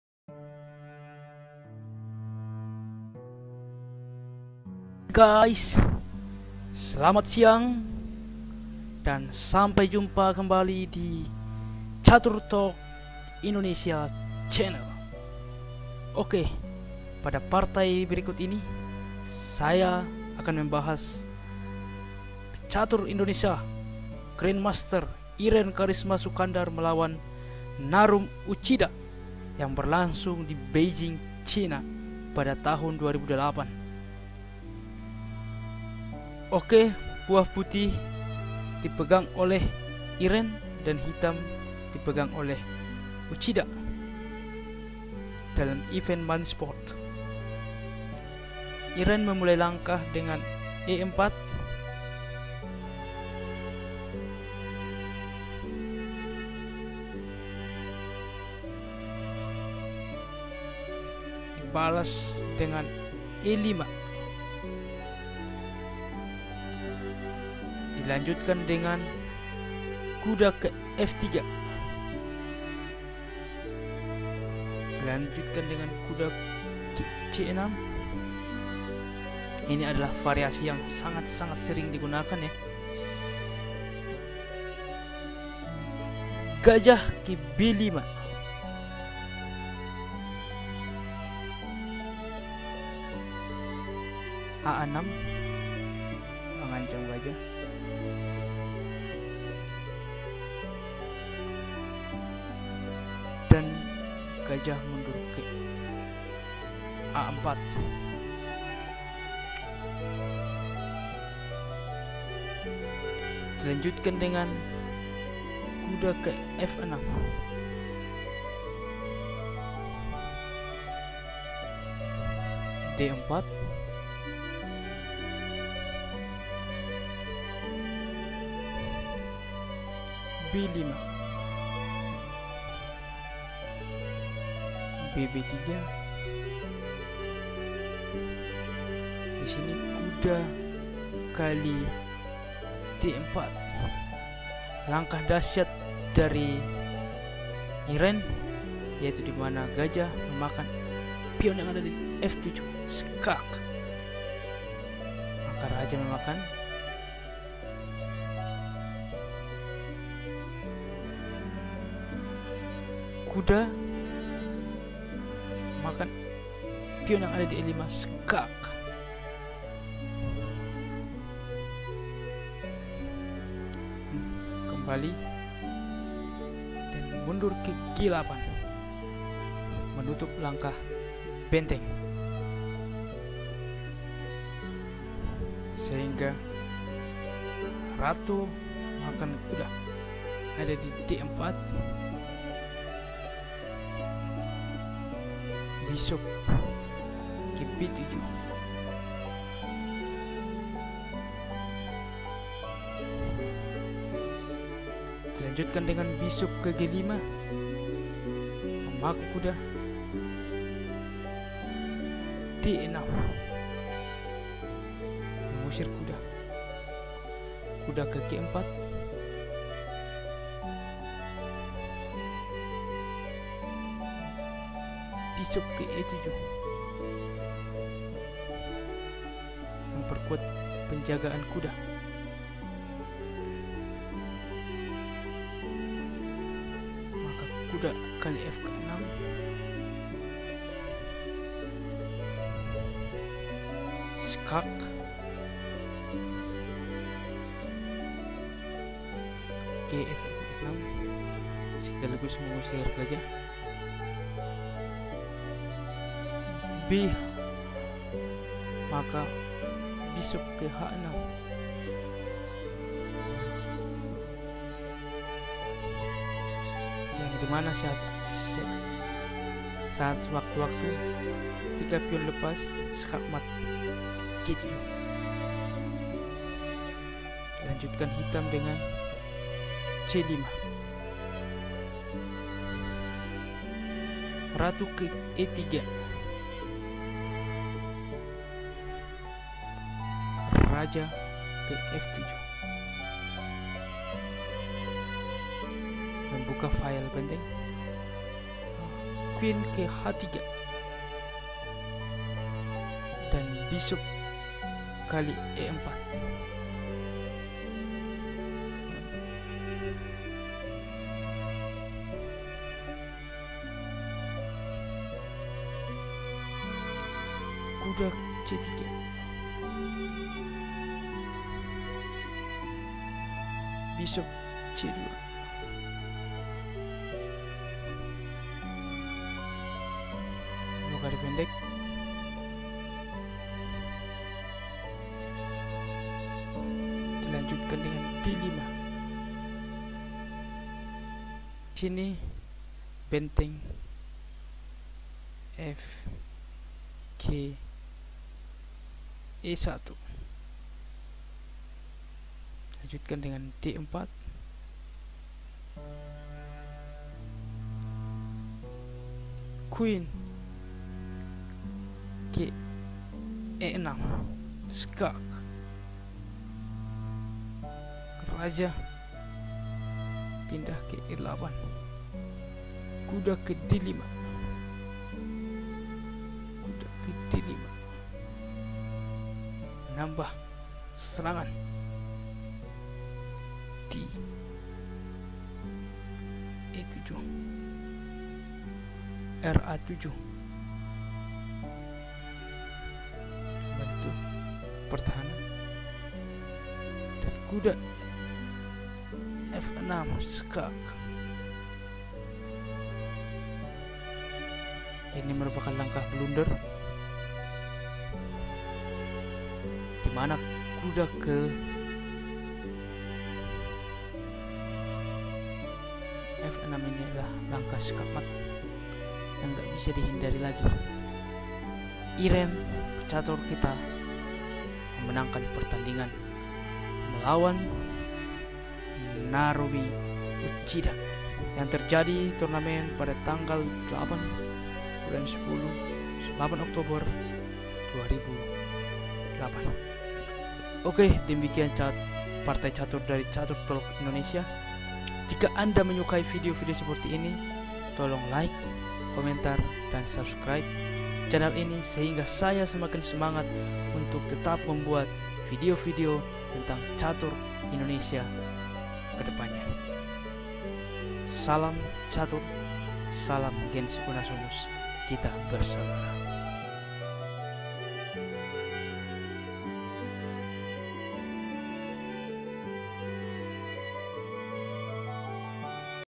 LANGKAH KUDA MEMATIKAN "Irene Kharisma Sukandar VS Narumi Uchida" CATUR TALK INDONESIA
Event "Mindsports" Beijing CHN, 8 Oktober 2008
Irine Kharisma Sukandar (PUTIH) dan Narumi Uchida (HITAM)
1-0 ECO "C77"

Berikut Partainya :
1. e4 e5 2. Nf3 Nc6 3. Bb5 a6 4. Ba4 Nf6 5. d4 b5
6. Bb3 Nxd4 7. Bxf7+ Kxf7 8. Nxe5+ Kg8 9. Qxd4 Bb7
10. Bg5 d6 11. Ng4 Be7 12. Nxf6+ gxf6 13. Bh6 c5
14. Qe3 Kf7 15. Qh3 Bxe4 16. Nc3 Bxc2 17. O-O d5
18. Rfe1 d4 19. Qe6+ Ke8 20. Nd5 Ra7 21. Nxf6+
1-0